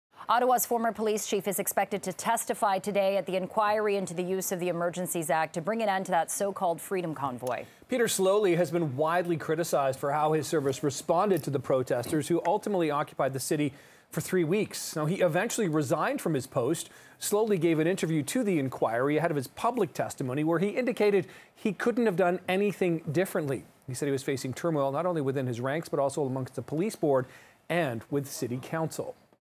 Former Ottawa police chief Peter Sloly to testify at Emergencies Act inquiry
Former Ottawa police chief Peter Sloly will testify today at the public inquiry into the federal government's use of the Emergencies Act to clear "Freedom Convoy" protesters.

Sloly resigned the day after Prime Minister Justin Trudeau announced the legislation would be triggered in February.